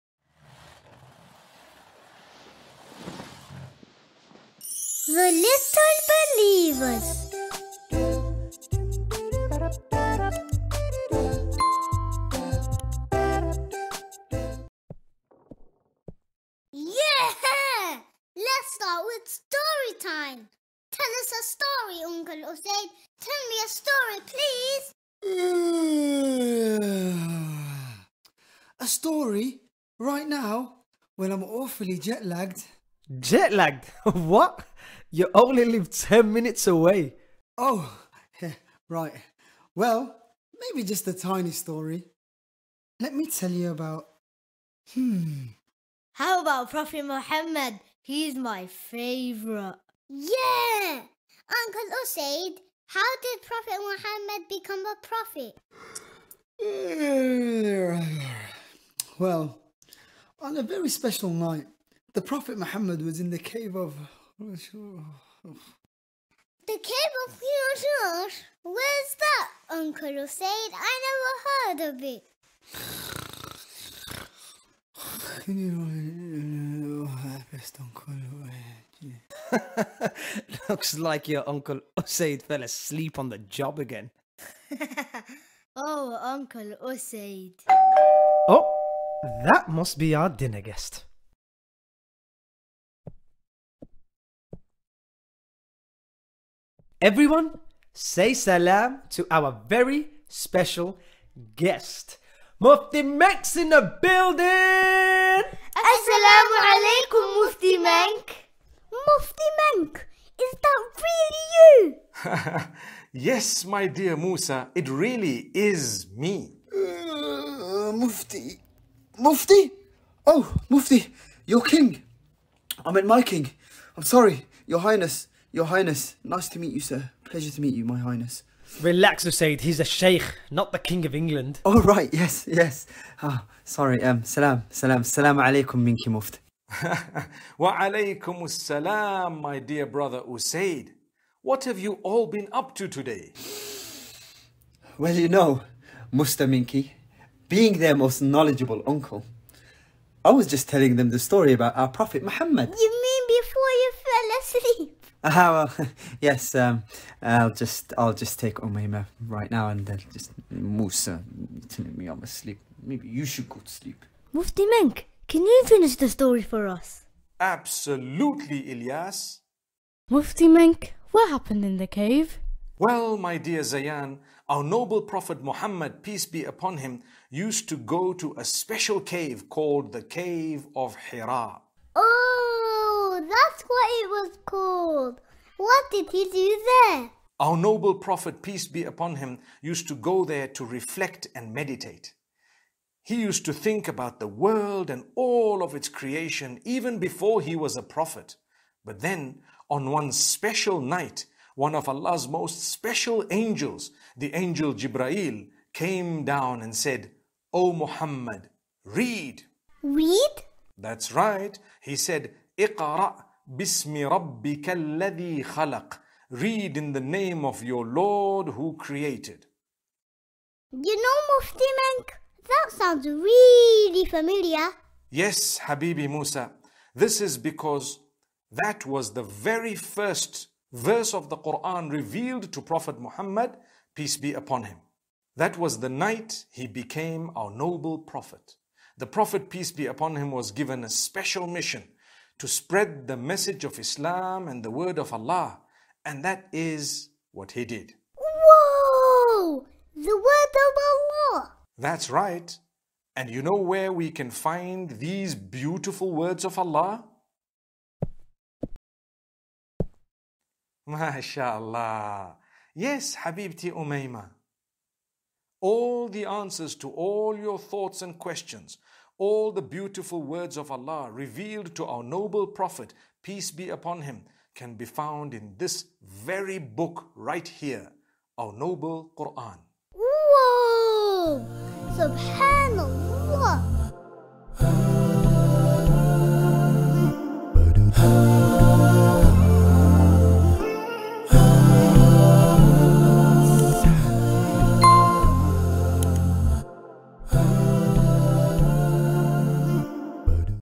TLB - The Revelation | Animated Story With Mufti Menk
*NO MUSICAL INSTRUMENTS WERE USED IN THIS VIDEO*

The one and only Mufti Menk joins The Little Believers family for an engaging story time special episode to enlighten the littlest hearts about the incomparable value of the most powerful night. Watch “The Revelation” with Mufti Menk as he narrates the miraculous story of how the Quran was revealed to our most Noble Prophet Muhammad (saw).

Script and Storyboard: Kazima Wajahat
Voiceover: Mufti Menk, Muad, Zayaan, Ilyas, Musa, Usayd